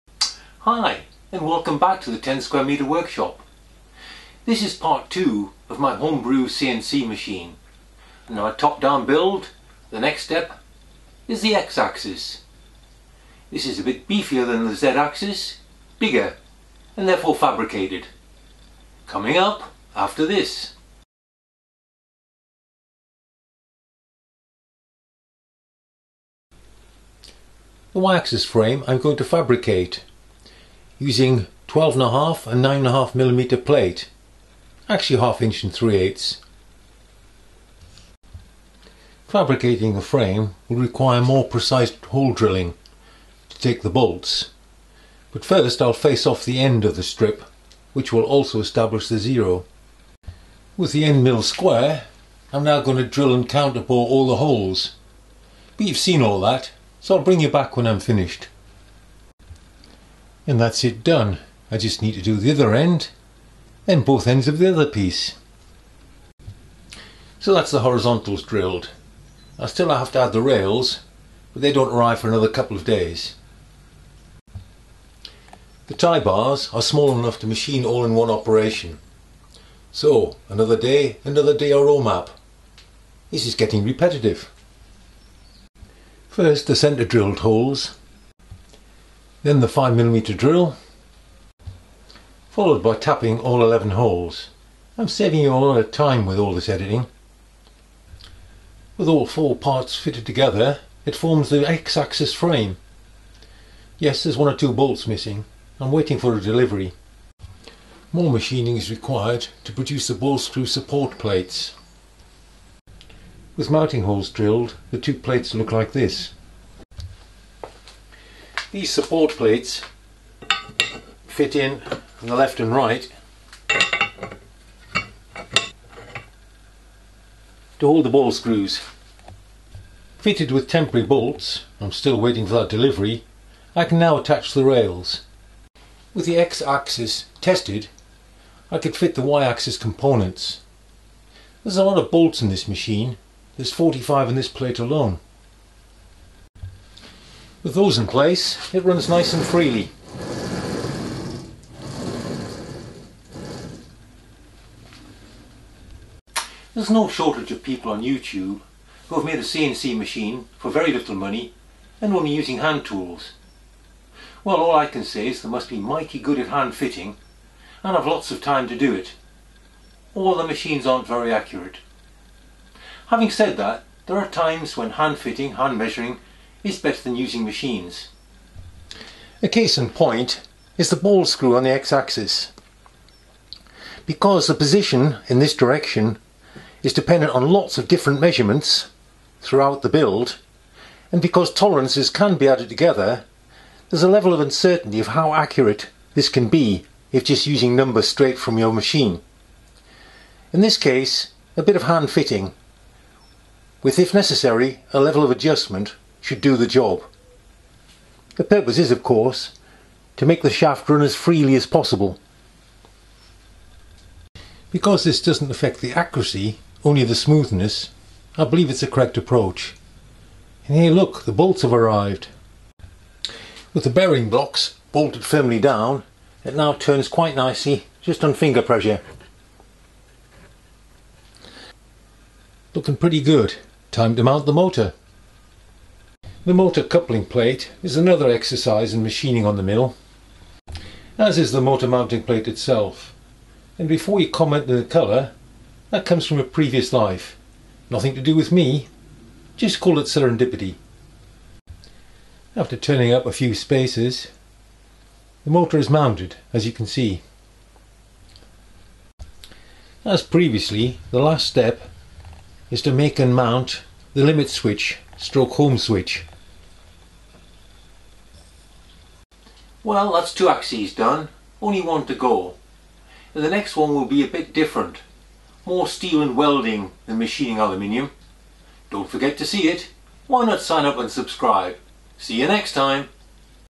Homebrew CNC part2
In this video I build the X axis mechanism. This carries the Z axis built last time, and is the major part of the machine.